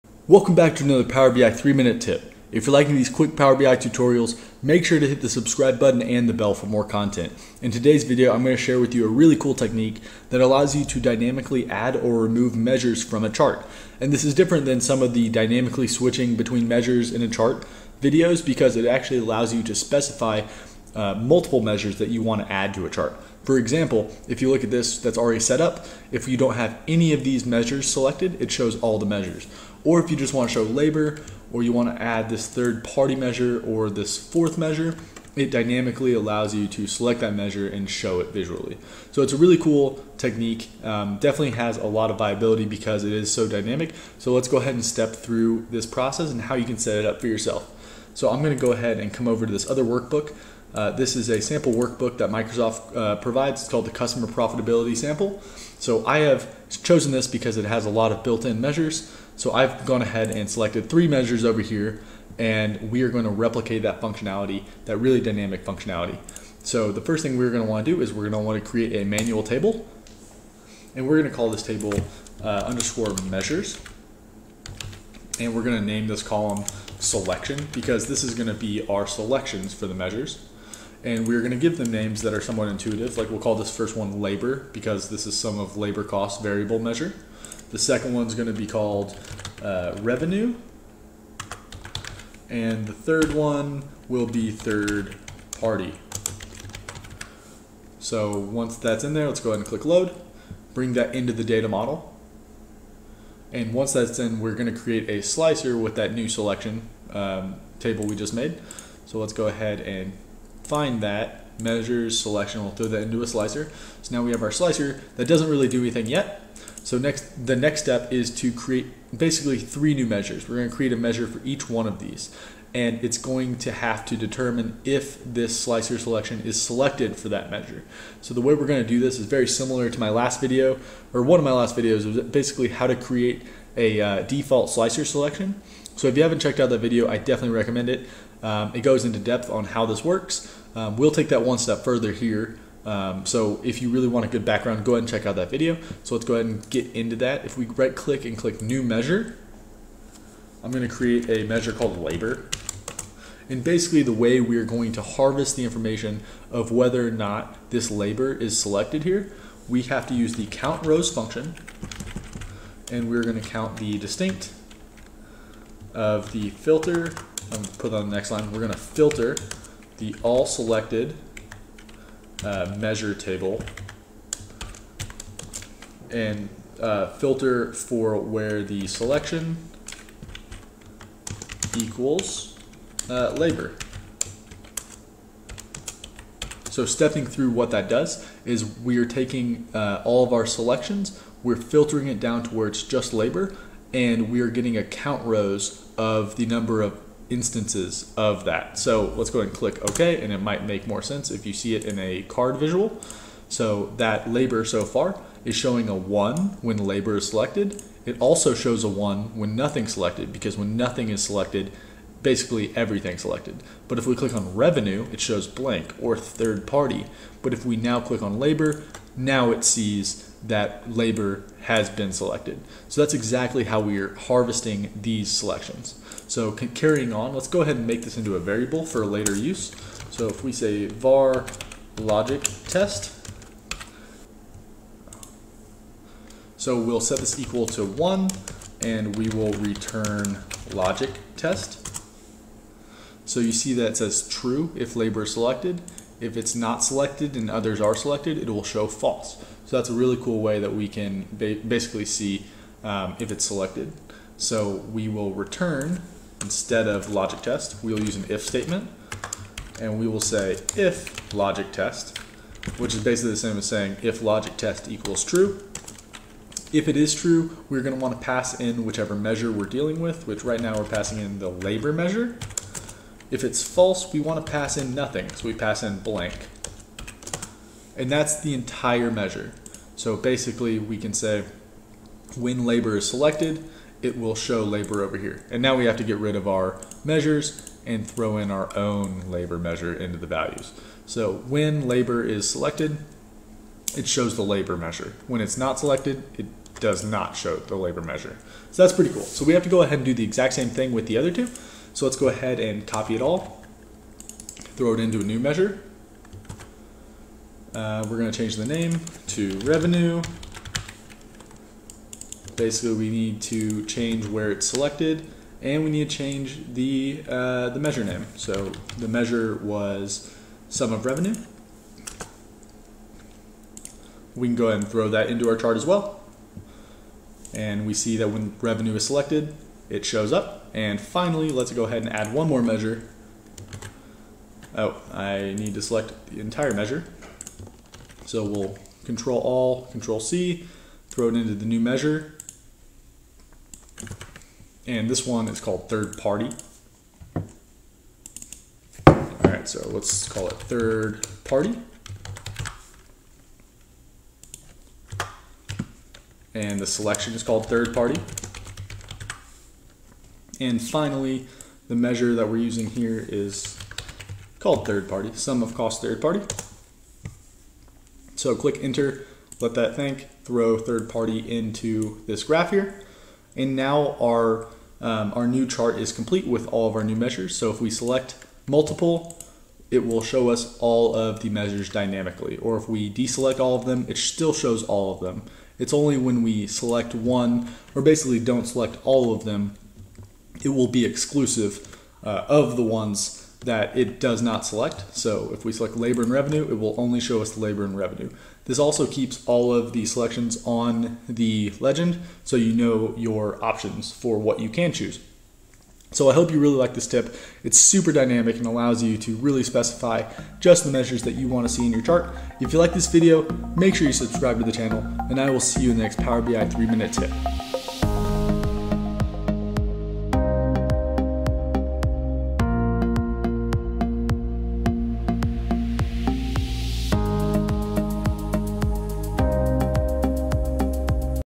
Power BI - Dynamically Add or Remove Measures from Visual
In this video, I show you how to dynamically set multiple measures in a chart! You can add or remove these measures simply by selecting them!